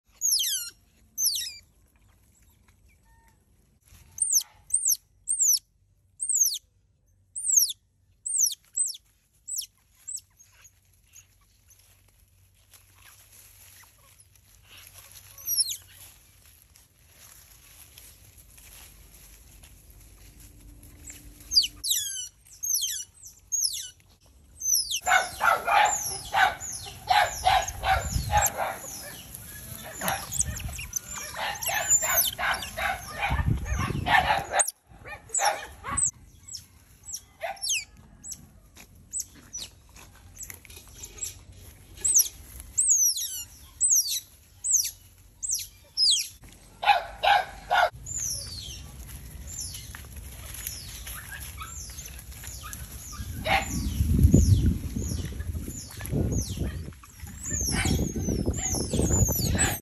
Otter Parents Send Their Babies To Me
Sea creatures are being overfished by electric fishing boats in the mangroves. Otter Tipo helps wild otters come to me for food and shelter.